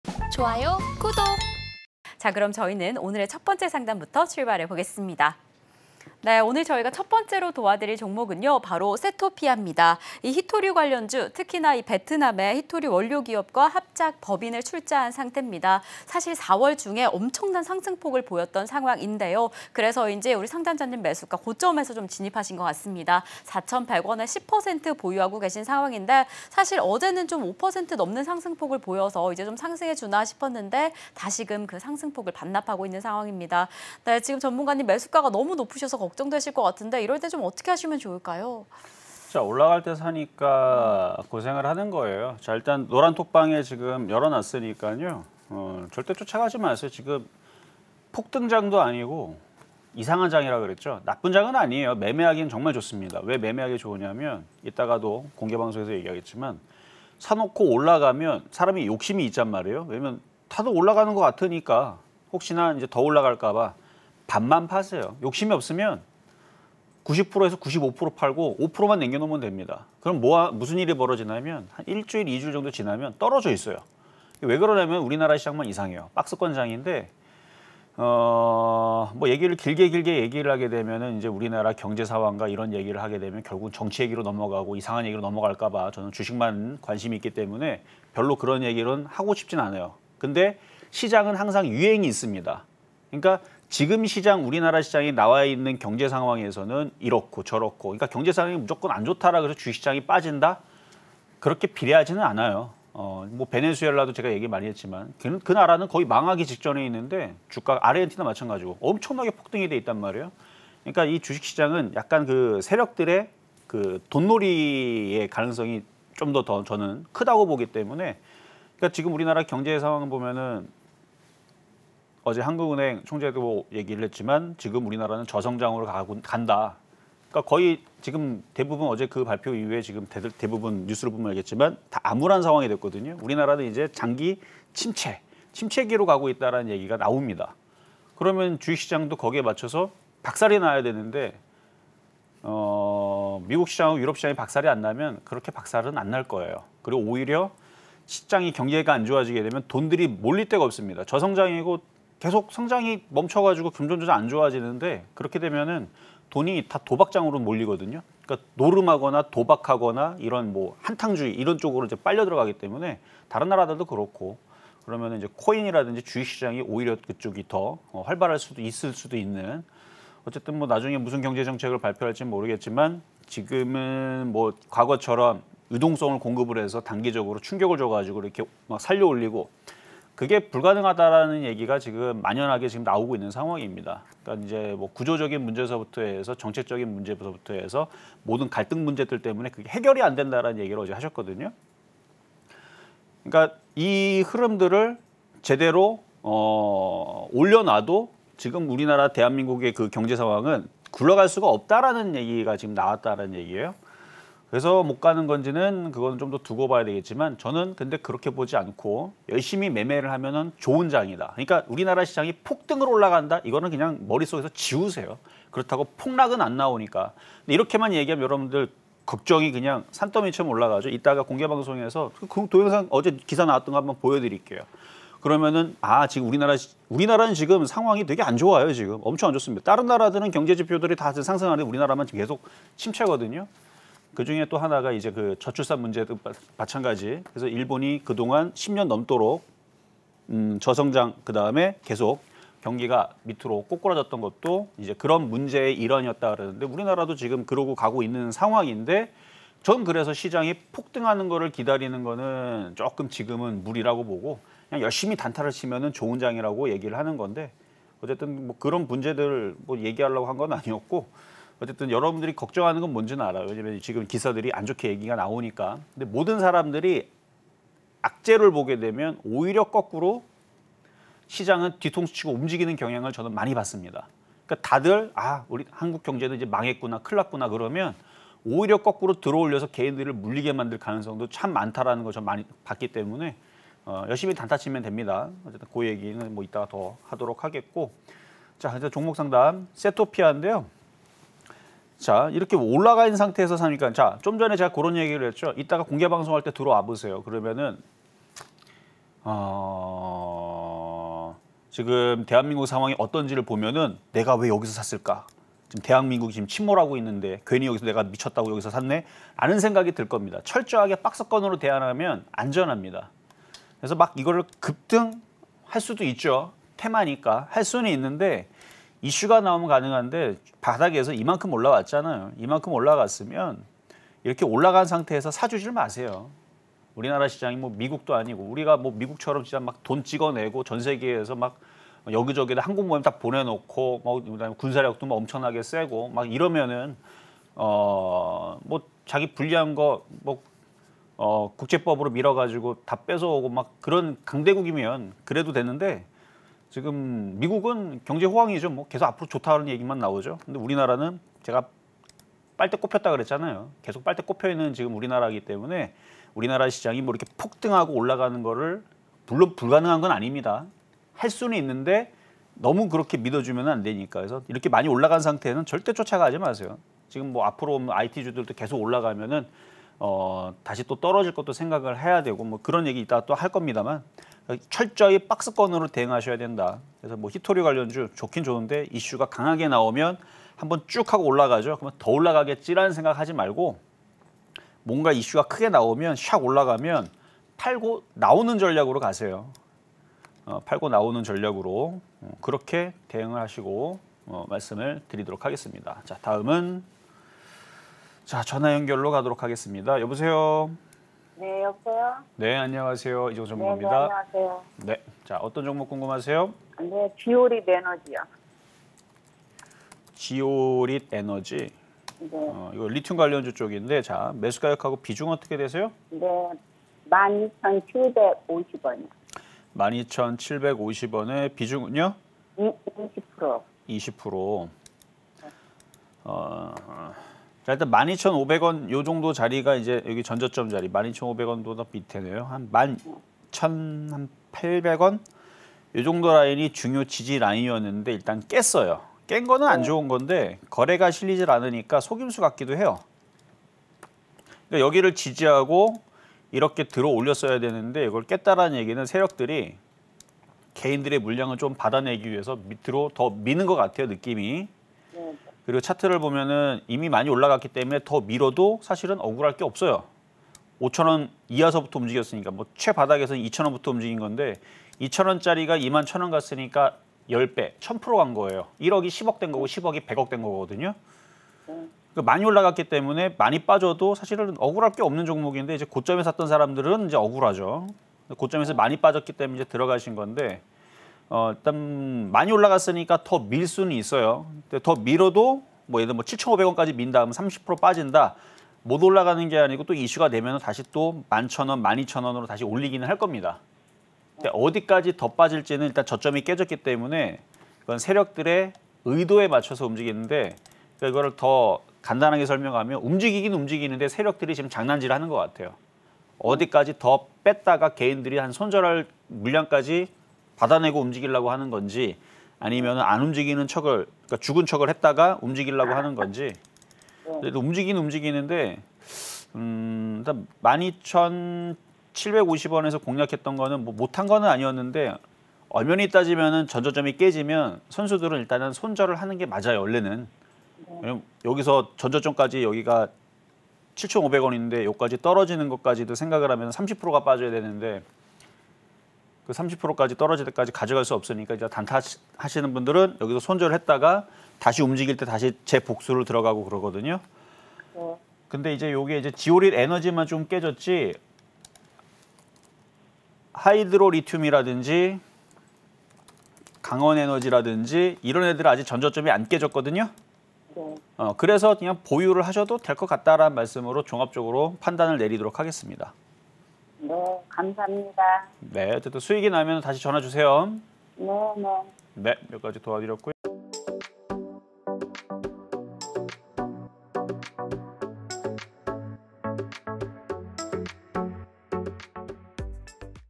[종목상담] 세토피아, 지오릿에너지 │ 20230531 실전매매의 정수
종목명 : 세토피아 (222810) / 매수가 : 4,100원 / 비중 : 10%
종목명 : 지오릿에너지 (270520) / 매수가 : 12,750원 / 비중 : 15%